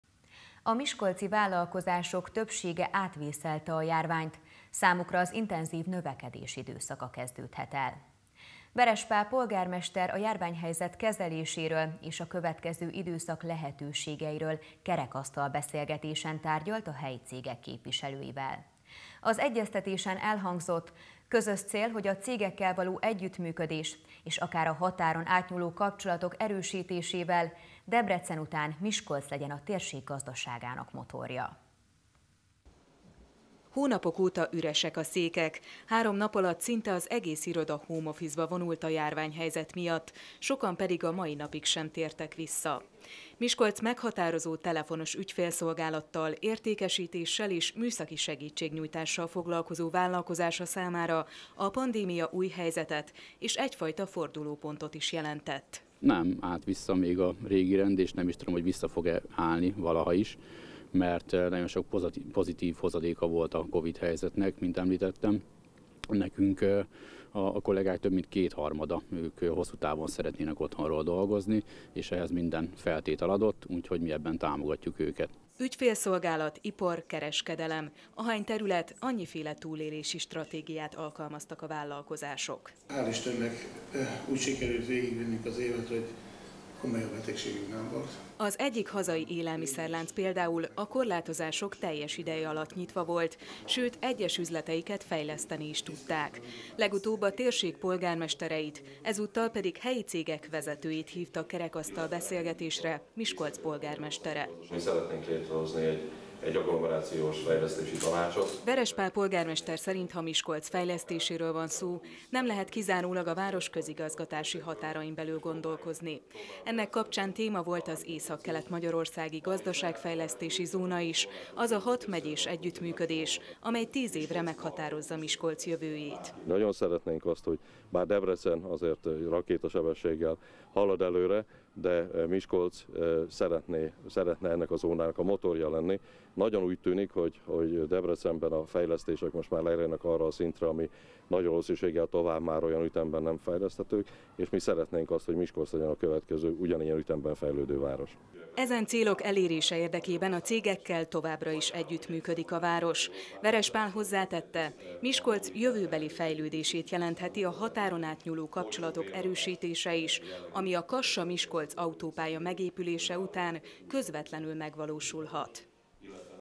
Együtt a fejlődésért
A miskolci vállalkozások többsége átvészelte a járványt, számukra az intenzív növekedés időszaka kezdődhet el. Veres Pál, polgármester a járványhelyzet kezeléséről és a következő időszak lehetőségeiről kerekasztal beszélgetésen tárgyalt a helyi cégek képviselőivel.